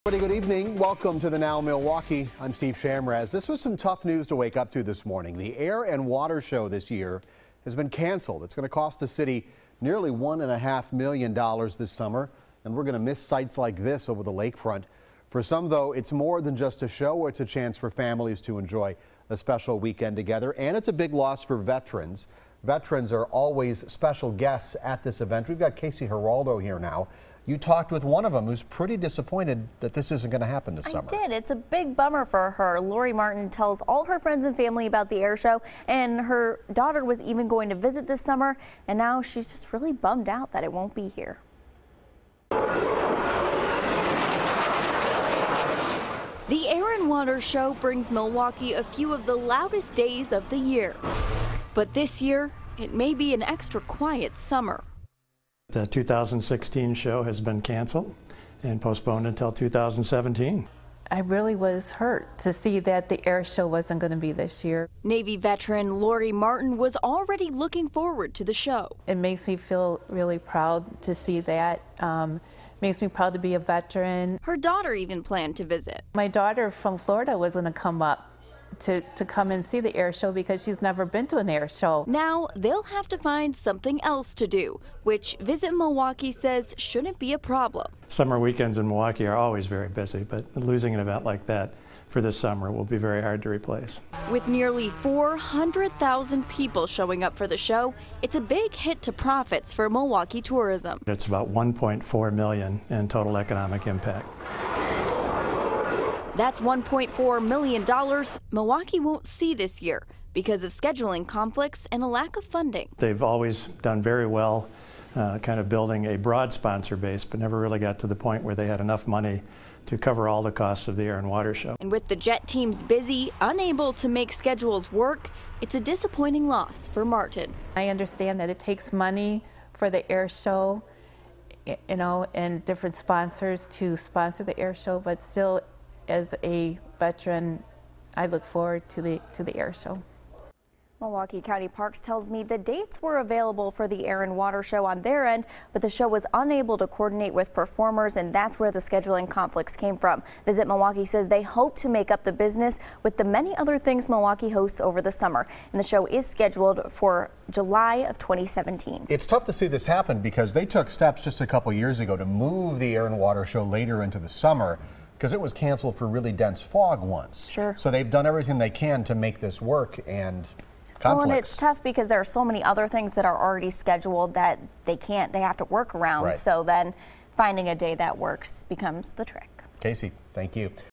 Veteran disappointed by Air Show's cancellation
It's an event that often celebrates veterans, even benefiting Stars and Stripes Honor Flight.